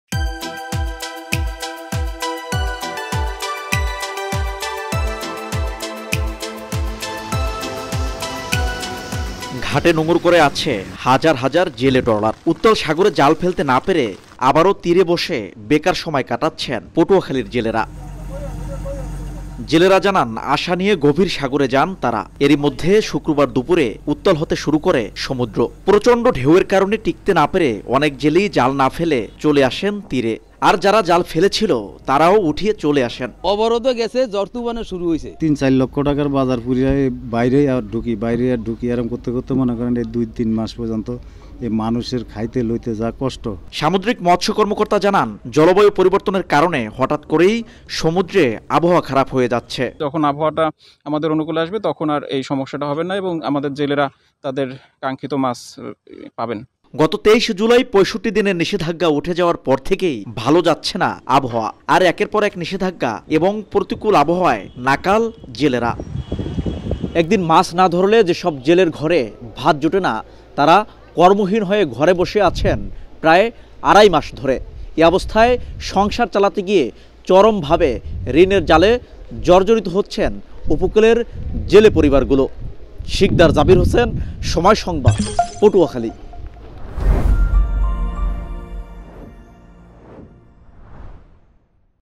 উত্তাল সমুদ্রে টিকতে পারছে না জেলেরা | Fisherman | Patuakhali | Somoy TV
ফের উত্তাল সমুদ্র। সাগরে জাল ফেলে টিকতে না পারায় তীরে ফিরছেন জেলেরা। দুদিন ধরে ঘাটে নোঙর করে আছে হাজার হাজার জেলে ট্রলার। বৈরী আবহাওয়া আর নিষেধাজ্ঞায় নাকাল জেলেদের পথে বসার উপক্রম।